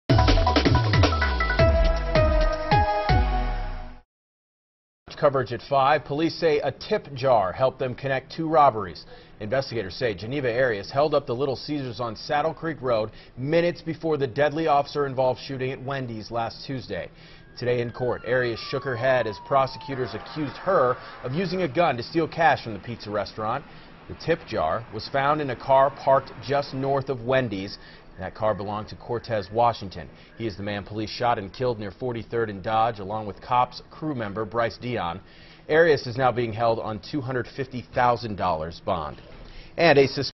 Tip jar stolen from Little Caesar's found near scene of officer-involved shooting
Bail is set for a woman accused in a robbery that happened just minutes before a fatal officer-involved shooting last week.